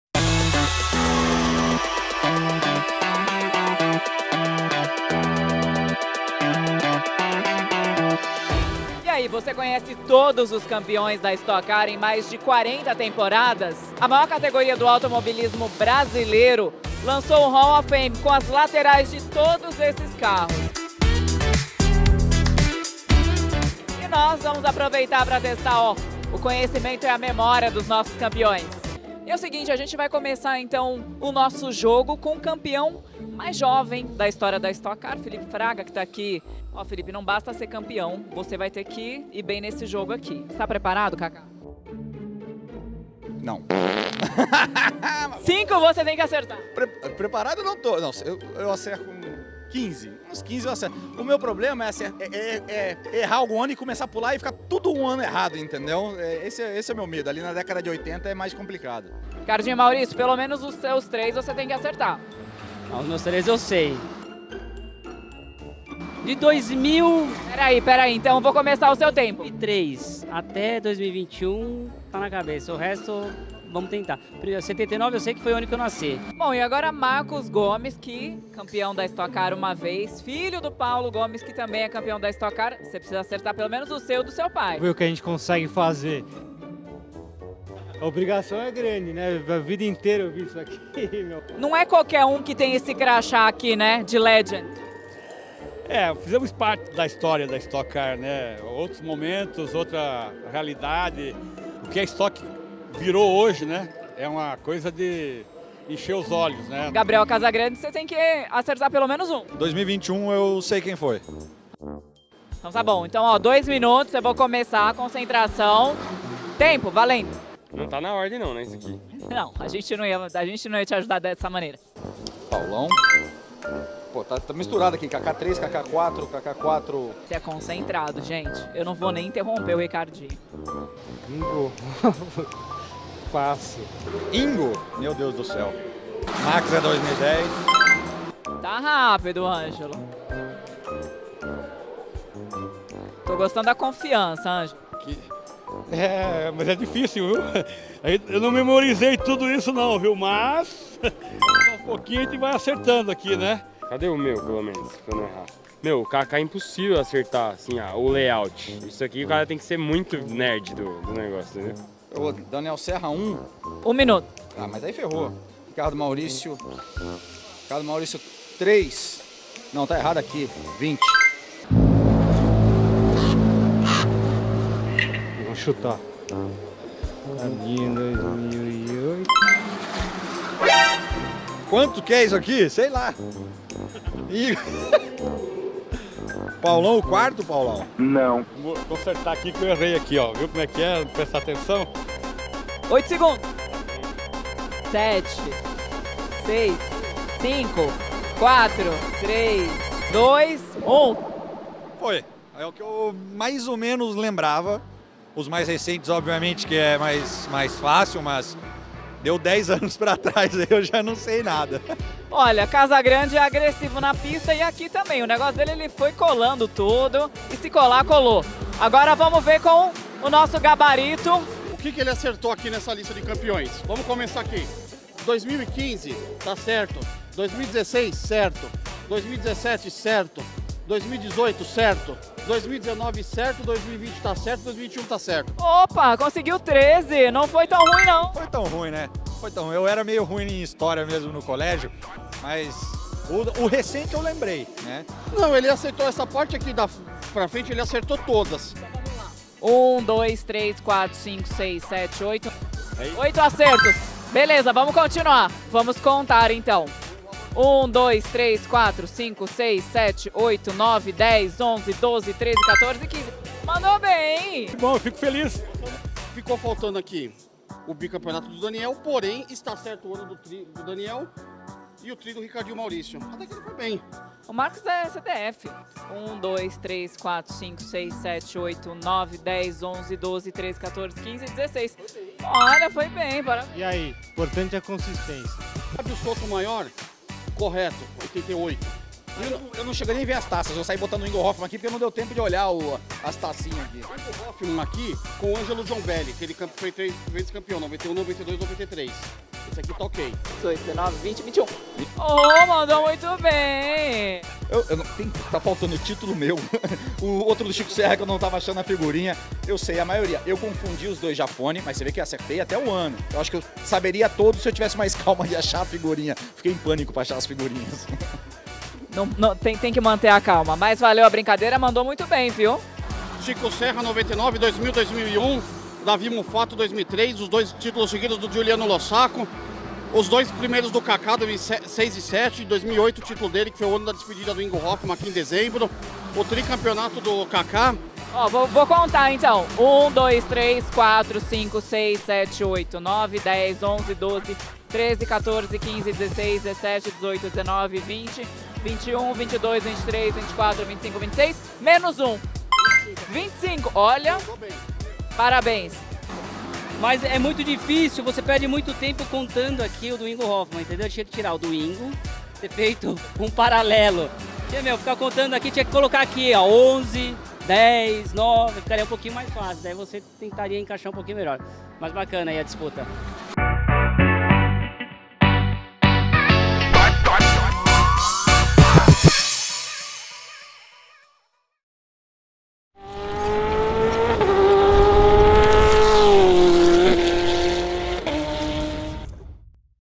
HALL OF FAME: VOCÊ CONHECE OS 43 CARROS CAMPEÕES DA STOCK CAR?
A Stock Car está lançando seu primeiro NFT, que chega como uma homenagem à história da maior categoria do automobilismo brasileiro. Aproveitamos a apresentação do pôster com as ilustrações dos 43 carros campeões de 1979 a 2021 para testarmos os conhecimentos de alguns dos nossos campeões. Eles tiveram dois minutos para encaixar as figurinhas nas respectivas temporadas. Quem será que levou a melhor?

As ilustrações foram desenvolvidas pelo designer Ararê Novaes com exclusividade para a Vicar Promoções Desportivas. Ao adquirir o pôster, você conquista muitos benefícios relacionados aos eventos da Stock Car Pro Series e ainda ajuda o Instituto Ingo Hoffmann.